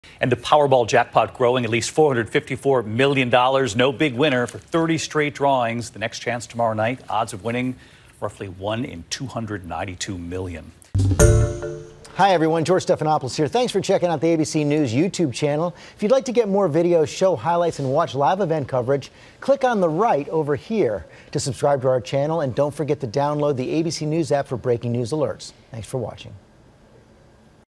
No big Powerball winner for 30 draws
After Monday night's lotto drawing there is still no big Powerball winner. The Jackpot is now $454 million and the next drawing is Wednesday.